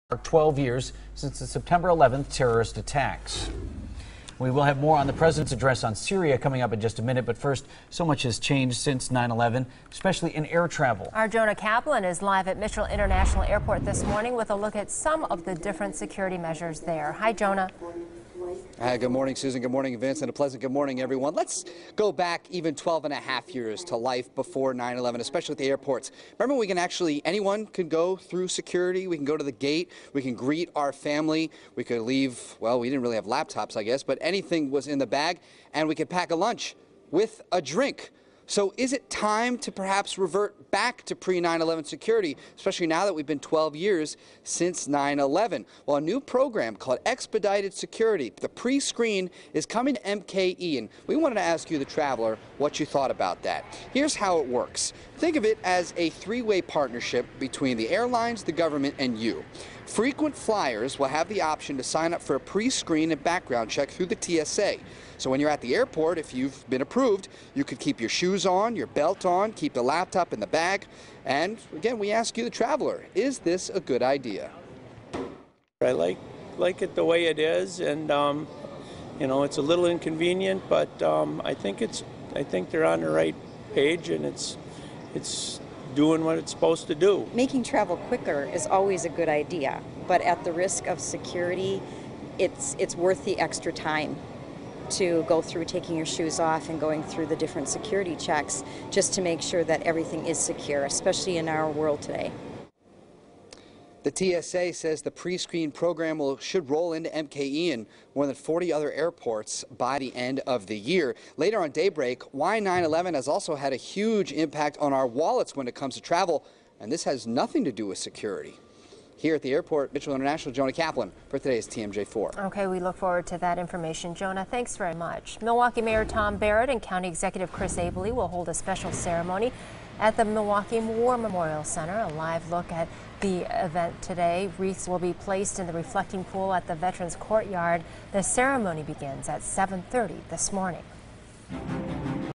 How security has changed since 9/11
Airport security has changed significantly since 9/11.